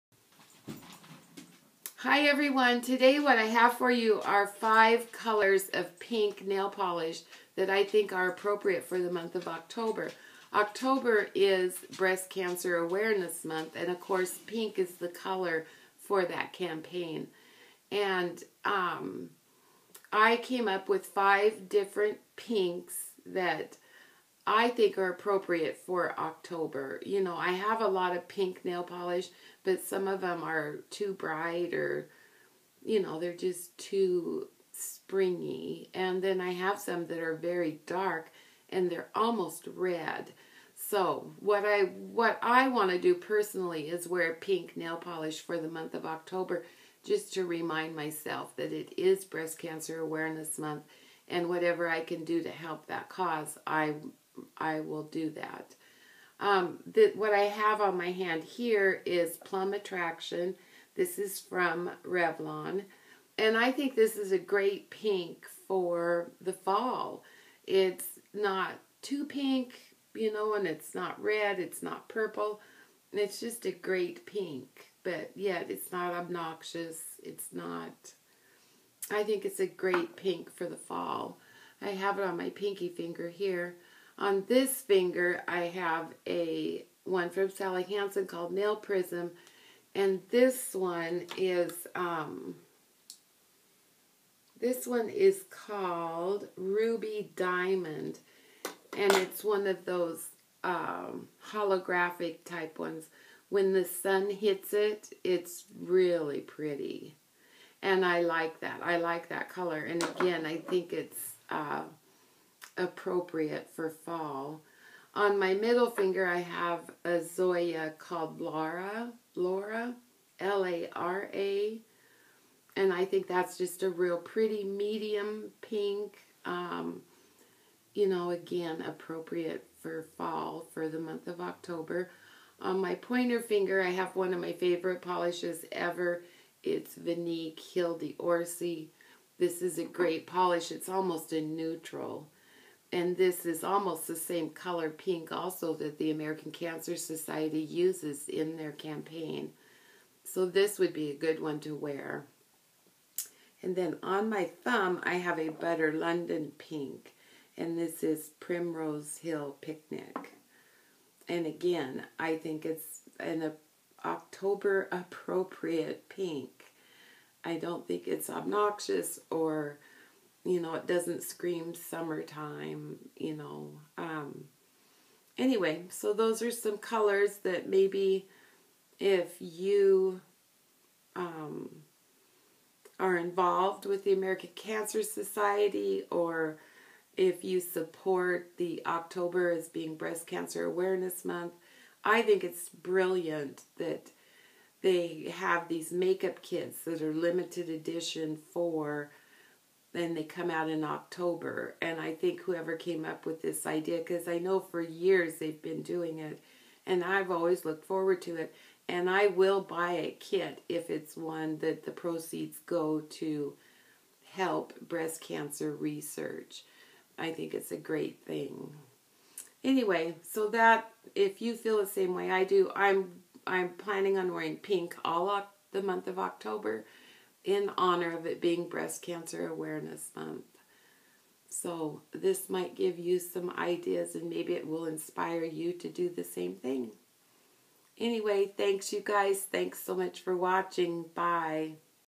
5 Shades of Pink for October's Breast Cancer Awareness Month
Of course, right off the bat I called my polish Plum Attraction and it is Plum Seduction by Revlon. I guess it wouldn't be a video from Judi without something being said wrong! Sorry! Plum SEDUCTION!

As you all know October is Breast Cancer Awareness Month and pink is the color of this campaign.  I am personally going to wear a pink nail polish every day this month in honor of this campaign and in honor of friends and loved ones I've lost to cancer.  Please contact the American Cancer Society if you have any questions or would like to see how you can help.